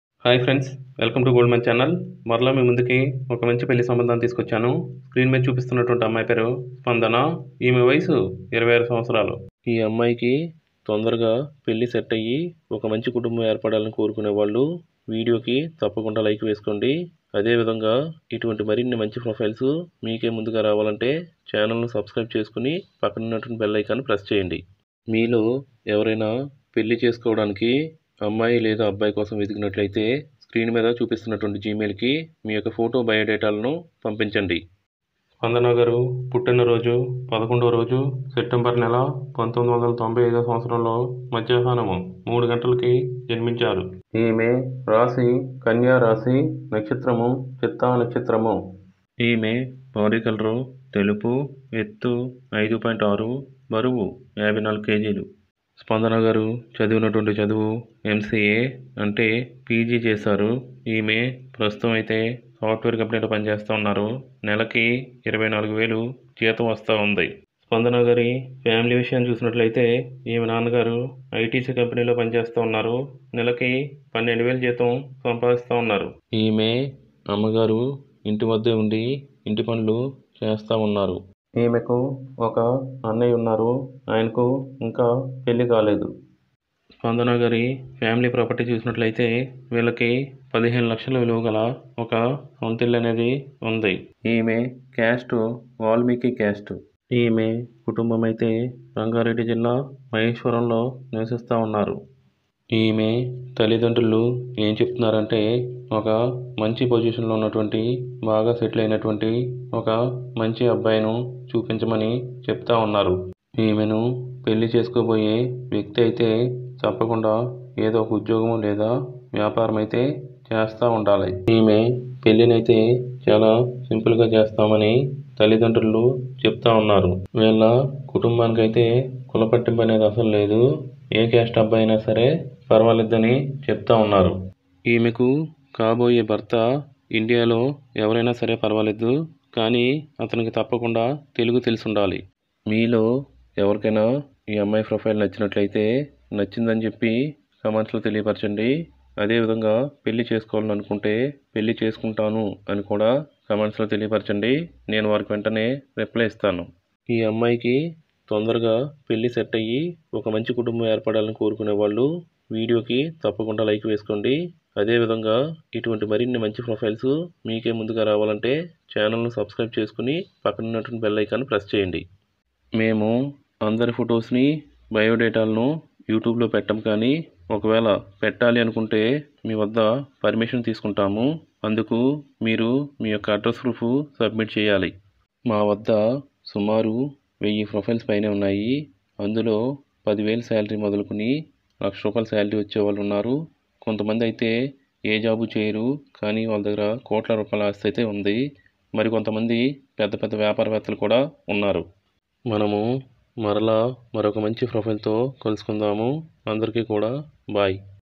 స్పందన కోసం పెళ్ళికొడుకు కావాలి | how to fill form from starting to ending
Update details in form in simple way
మరిన్ని పూర్తి వివరాలు ఈ క్రింది website లో ఉంటాయి.